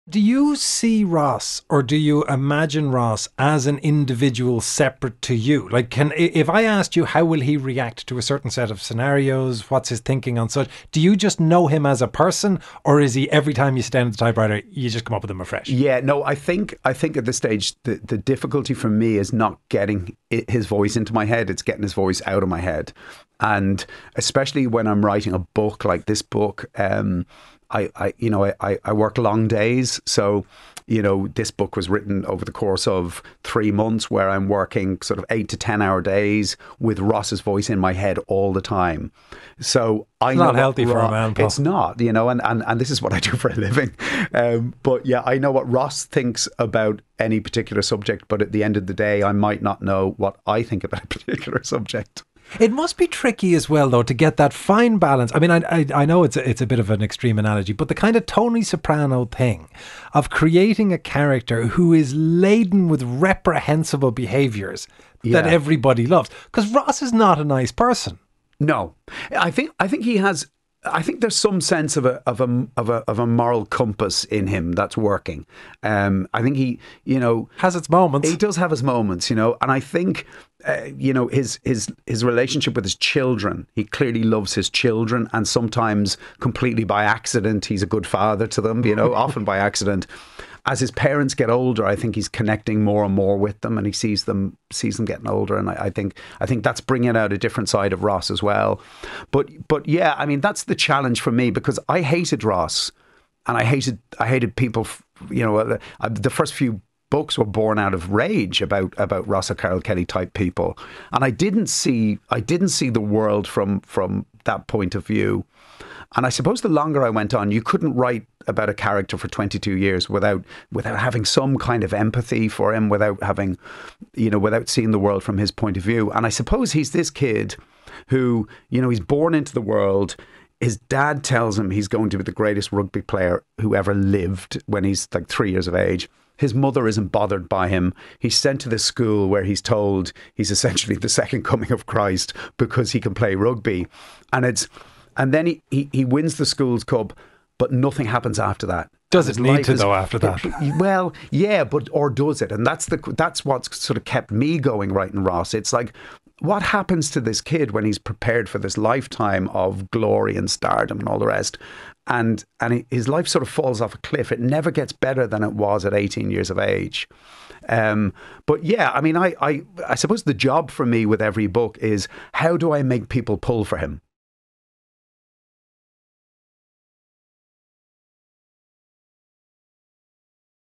Ross O'Carroll-Kelly: Can a horrible character be loved?
Paul Howard, creator of the Ross O'Carroll-Kelly series of books, joins Anton Savage to discuss his most famous creation taking up his brain space, and the struggle to make readers fall in love with a horrible character.